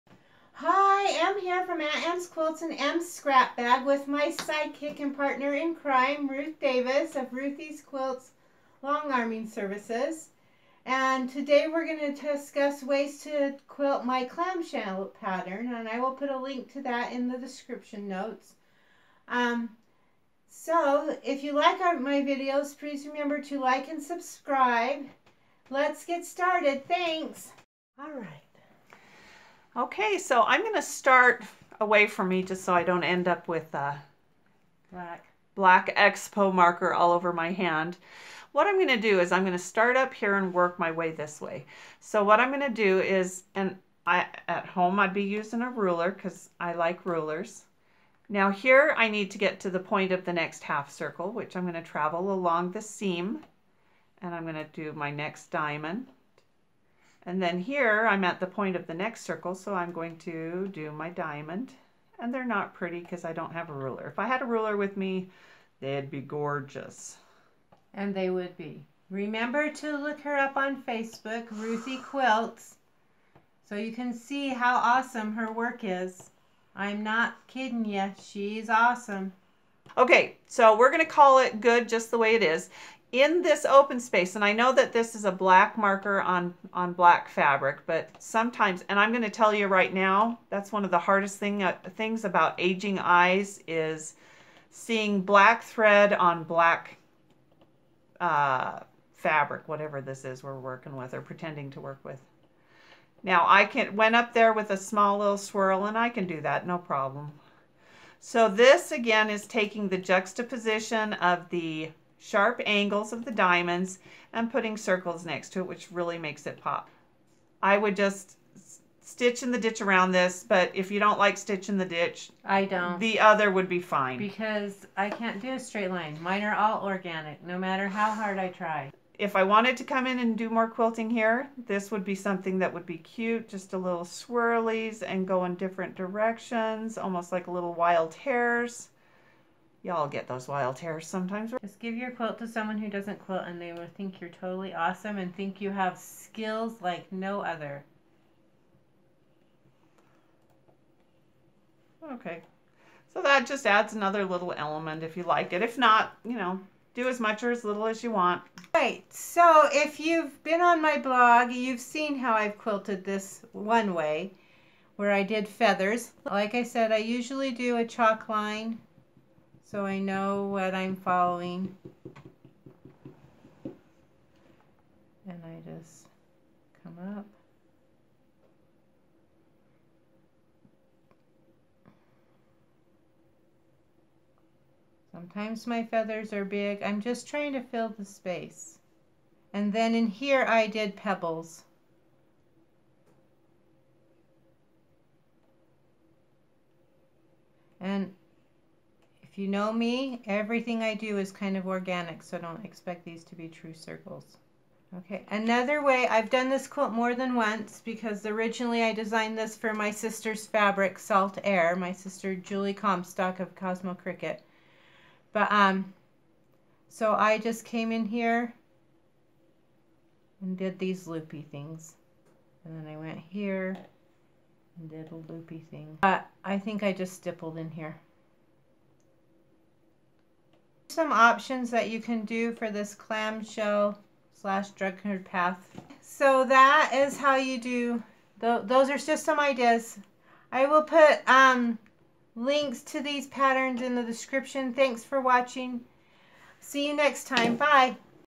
Free Motion Quilting In Curved Spaces
Join Ruth Davis and I as we share ways to free motion quilt a quilt with curves.